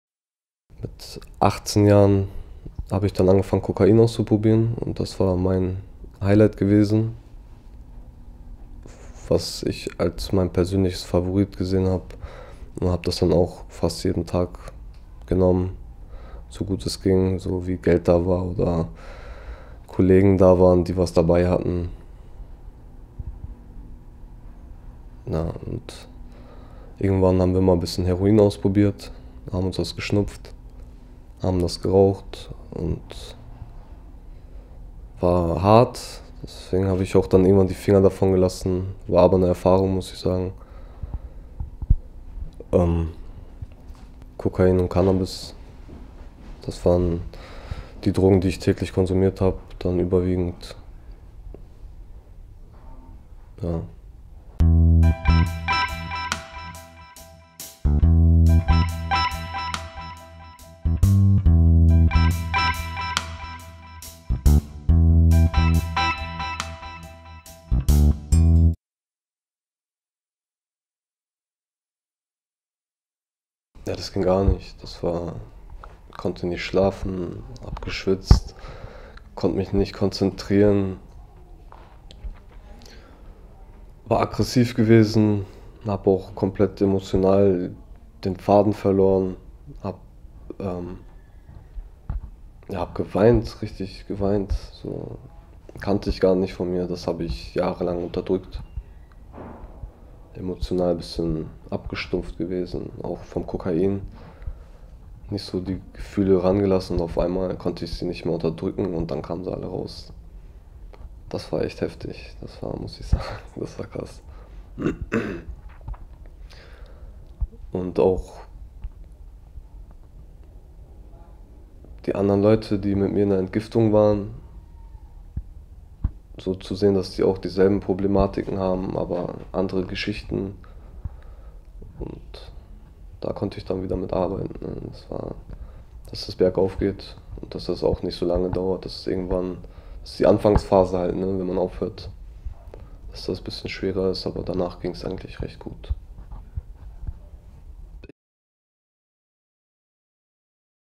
Cannabis, Kokain, Heroin: Osnabrücker erzählt von seiner Drogenkarriere (Teil 3/5: Der Entzug)
Tolgay ist Mitte 20. Cannabis konsumierte er das erste Mal im Alter von 12 Jahren, mit 18 Jahren folgte Kokain und auch von Heroin und Amphetaminen konnte er nicht die Finger lassen. Mit 20 Jahren machte er den ersten Entzug. In unserer fünfteiligen Serie erzählt er von seinem Leben und der Sucht. Im dritten Teil spricht er darüber, wie er den Entzug wahrgenommen hat.
Weil er mittlerweile jobbt und nicht will, dass seine derzeitigen Arbeitgeber ihn erkennen, möchte er anonym bleiben. Die Redaktion hat daher seinen Namen und seine Stimme verändert.